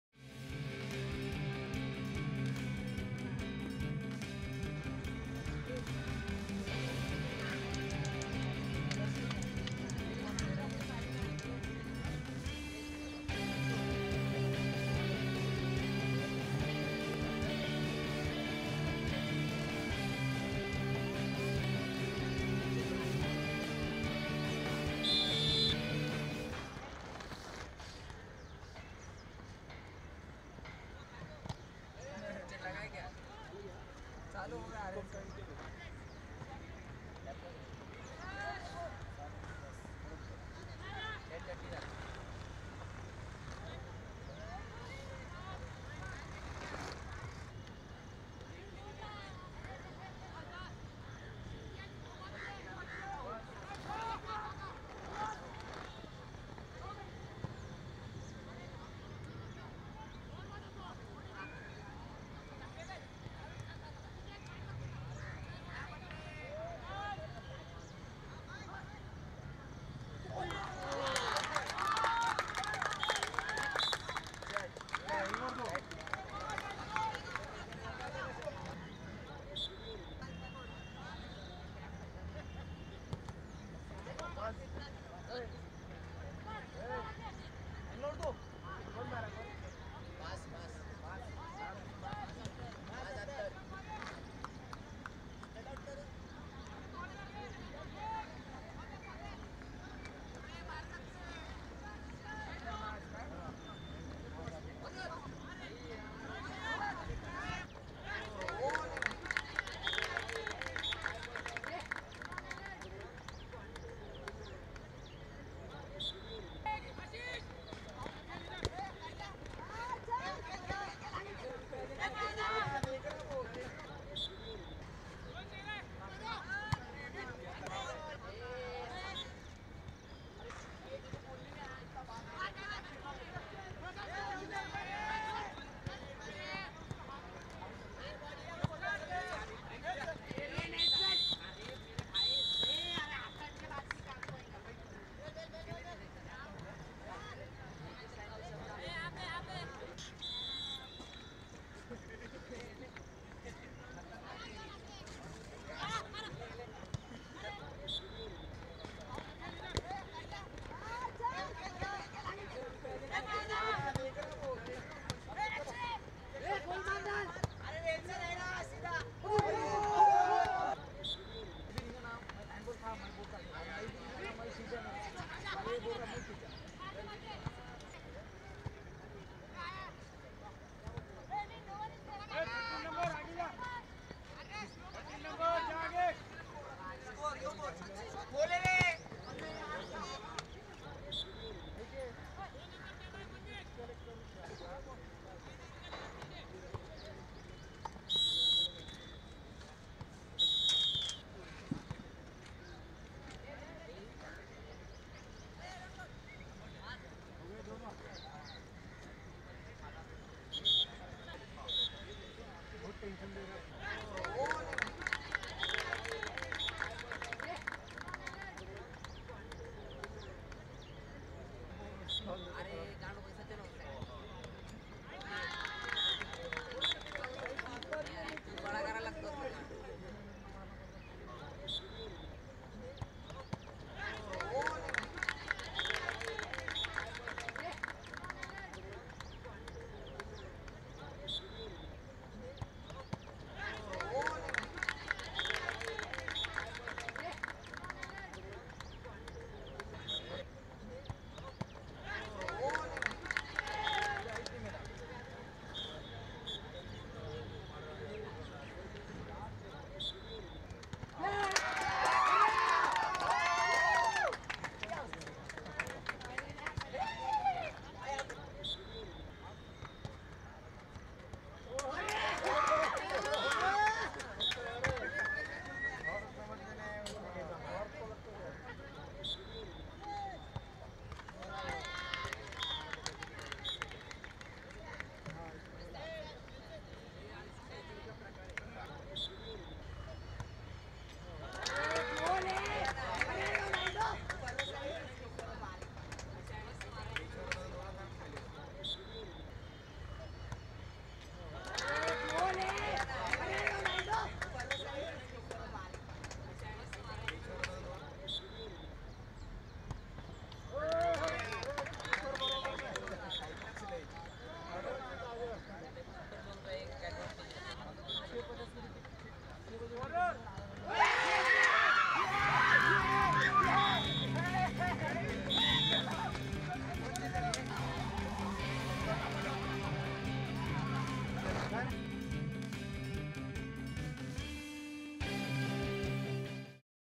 RFYS: Pune College Boys - Anglo Urdu Boys High School Vs Arihant College Highlights
Watch the match highlights of RFYS: Pune College Boys - Anglo Urdu Boys High School Vs Arihant College match held on 12th October, 2016.